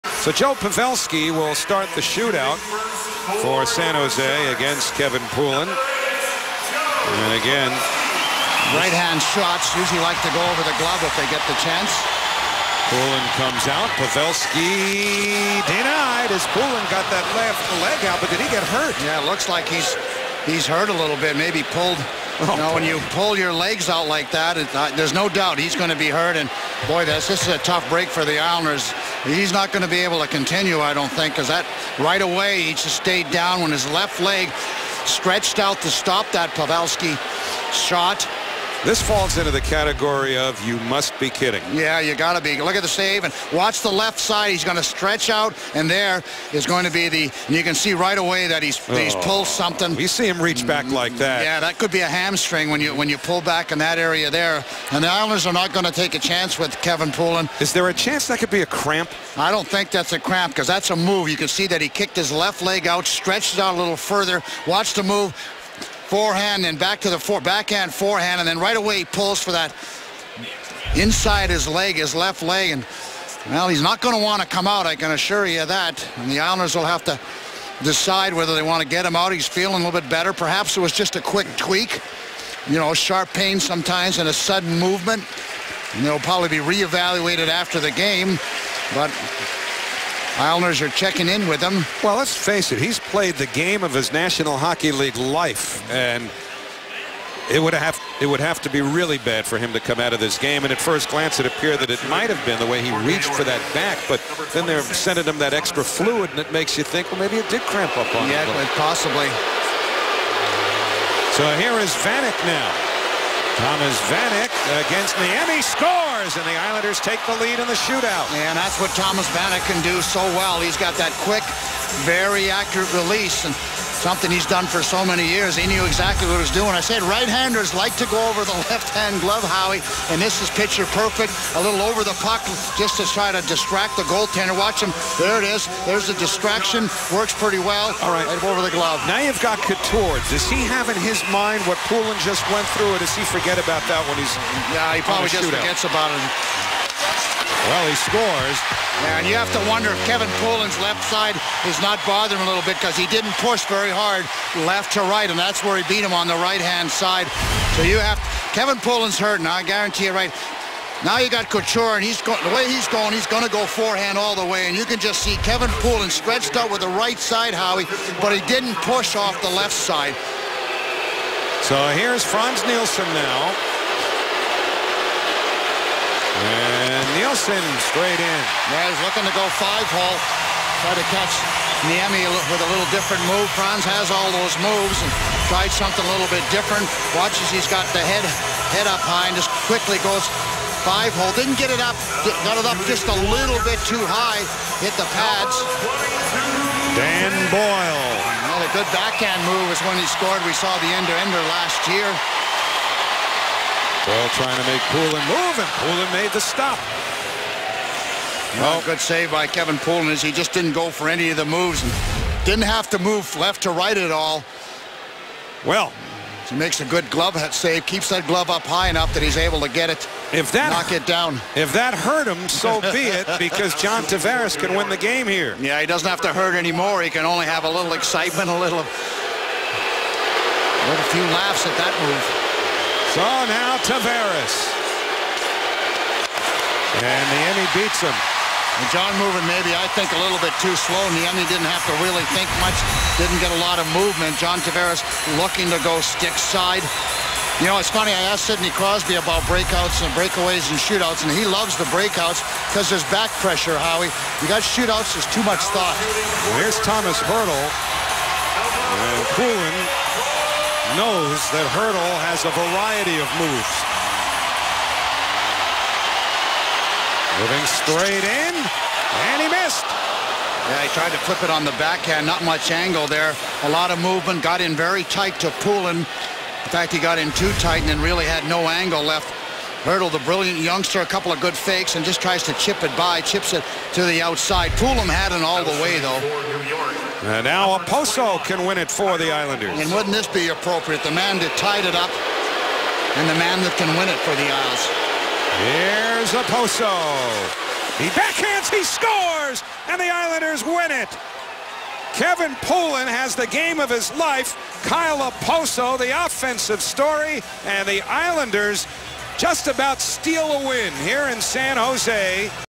Shootout: Islanders vs Sharks
Watch the shootout between the New York Islanders and the San Jose Sharks on 12/10/13.